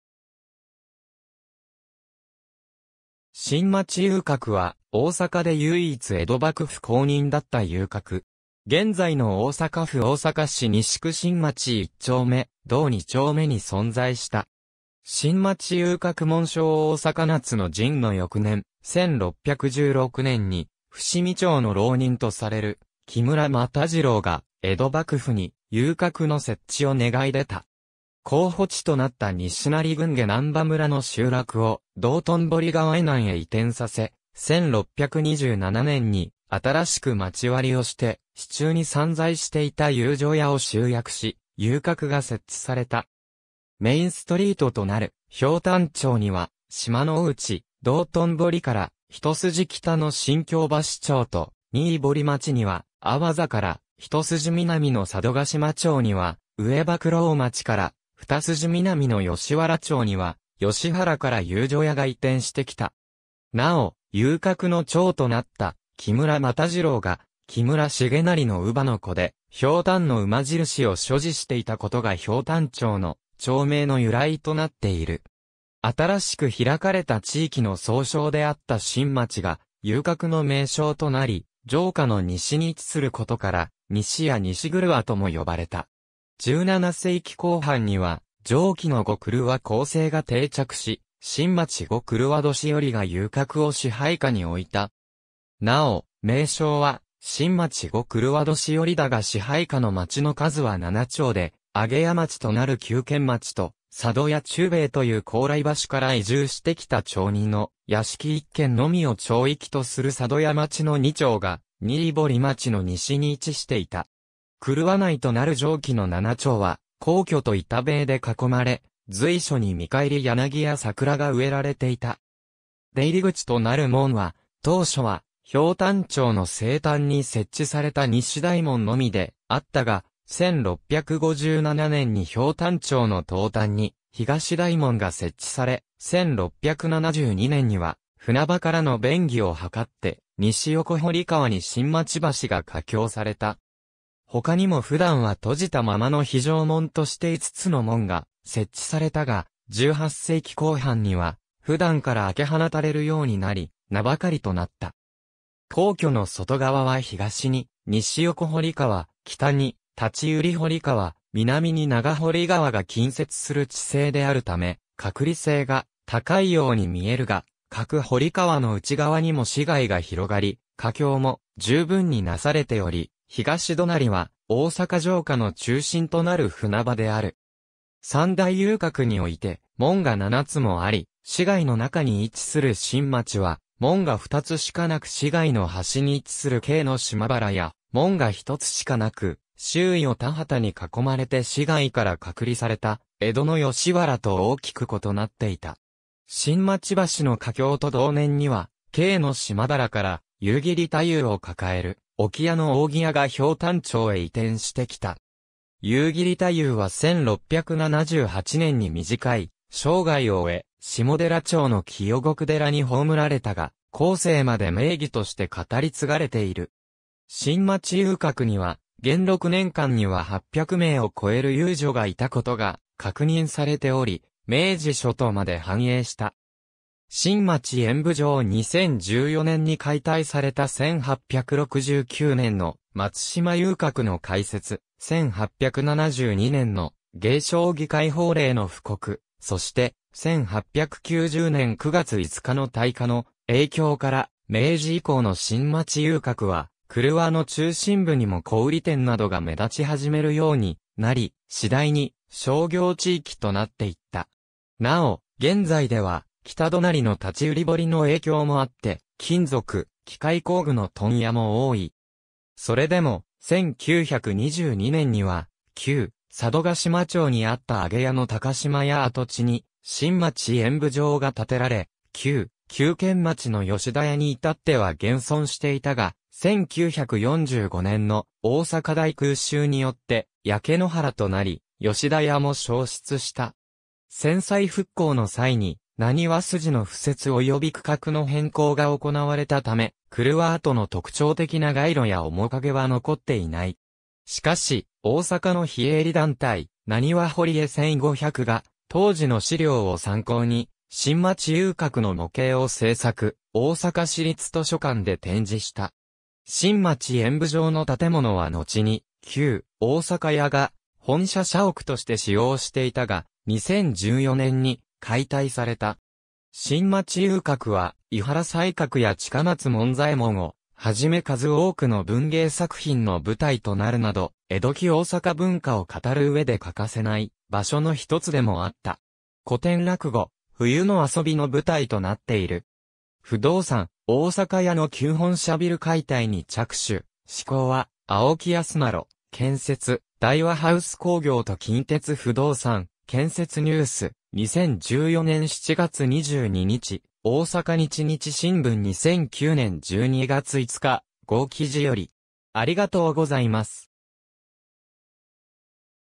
新町遊廓
新町遊廓（しんまちゆうかく）は、大坂で唯一江戸幕府公認だった遊廓（花街）。
現在の大阪府大阪市西区新町1丁目・同2丁目に存在した。
新町遊廓 紋章 大坂夏の陣の翌年、1616年（元和2年）に伏見町の浪人とされる木村又次郎が江戸幕府に遊廓の設置を願い出た。
候補地となった西成郡下難波村の集落を道頓堀川以南へ移転させ、1627年（寛永4年）に新しく町割をして市中に散在していた遊女屋を集約し、遊廓が設置された。
メインストリートとなる瓢箪町（ひょうたんまち）には島之内・道頓堀から、1筋北の新京橋町（しんきょうばしちょう）と新堀町（しんぼりちょう）には阿波座から、1筋南の佐渡島町（さどじまちょう）には上博労町（現・新町4丁目の木津川沿い）から、2筋南の吉原町（よしわらちょう）には葭原（天満北部）から遊女屋が移転してきた。
なお、遊廓の長となった木村又次郎が木村重成の乳母の子で、瓢箪の馬印を所持していたことが瓢箪町の町名の由来となっている。
新しく拓かれた地域の総称であった新町が遊廓の名称となり、城下の西に位置することからニシや西廓とも呼ばれた。
17世紀後半には上記の五曲輪（くるわ）構成が定着し、新町五曲輪年寄が遊廓を支配下においた。
なお、名称は新町五曲輪年寄だが支配下の町の数は7町で、揚屋町となる九軒町（くけんちょう）と、佐渡屋忠兵衛という高麗橋から移住してきた町人の屋敷1軒のみを町域とする佐渡屋町（さどやまち）の2町が新堀町の西に位置していた。
廓内となる上記の7町は溝渠と板塀で囲まれ、随所に見返り柳や桜が植えられていた。
出入口となる門は、当初は瓢箪町の西端に設置された西大門のみであったが、1657年（明暦3年）に瓢箪町の東端に東大門が設置され、1672年（寛文12年）には船場からの便宜をはかって西横堀川に新町橋が架橋された。
他にも普段は閉じたままの非常門として5つの門が設置されたが、18世紀後半には普段から開け放たれるようになり、名ばかりとなった。
溝渠の外側は東に西横堀川、北に立売堀川、南に長堀川が近接する地勢であるため、隔離性が高いように見えるが、各堀川の内側にも市街が広がり、架橋も十分になされており、東隣は大坂城下の中心となる船場である。
三大遊廓において、門が7つもあり、市街の中に位置する新町は、門が2つしかなく市街の端に位置する京の島原や、門が1つしかなく周囲を田畑に囲まれて市街から隔離された江戸の吉原と大きく異なっていた。
新町橋の架橋と同年には、京の島原から夕霧太夫を抱える置屋の扇屋が瓢箪町へ移転して来た。
夕霧太夫は1678年（延宝6年）に短い生涯を終え、下寺町の浄国寺に葬られたが、後世まで名妓として語り継がれている。
新町遊廓には元禄年間には800名を超える遊女（太夫など）がいたことが確認されており、明治初頭まで繁栄した。
新町演舞場（旧・大阪屋本社社屋） 2014年に解体された 1869年（明治2年）の松島遊廓の開設、1872年（明治5年）の芸娼妓解放令の布告、そして、1890年（明治23年）9月5日の大火（新町焼）の影響から、明治以降の新町遊廓は廓の中心部にも小売店などが目立ち始めるようになり、次第に商業地域となっていった。
なお、現在では北隣の立売堀の影響もあって金属・機械工具の問屋も多い。
それでも、1922年（大正11年）には旧・佐渡島町にあった揚屋の高島屋跡地に新町演舞場が建てられ、旧・九軒町の吉田屋に至っては現存していたが、1945年（昭和20年）の大阪大空襲によって焼け野原となり、吉田屋も焼失した。
戦災復興の際になにわ筋の敷設および区画の変更が行われたため、廓跡の特徴的な街路や面影は残っていない。
しかし、大阪の非営利団体「なにわ堀江1500」が当時の資料を参考に新町遊廓の模型を制作、大阪市立図書館で展示した。
新町演舞場の建物はのちに旧・大阪屋が本社社屋として使用していたが、2014年（平成26年）に解体された。
新町遊廓は、井原西鶴や近松門左衛門をはじめ数多くの文芸作品の舞台となるなど、江戸期大坂文化を語る上で欠かせない場所のひとつでもあった。
古典落語「冬の遊び」の舞台となっている。
^ 【不動産】大阪屋の旧本社ビル解体に着手／施工は青木あすなろ建設／大和ハウス工業と近鉄不動産、建設ニュース、2014年7月22日 ^ 大阪日日新聞 2009年12月５日号記事より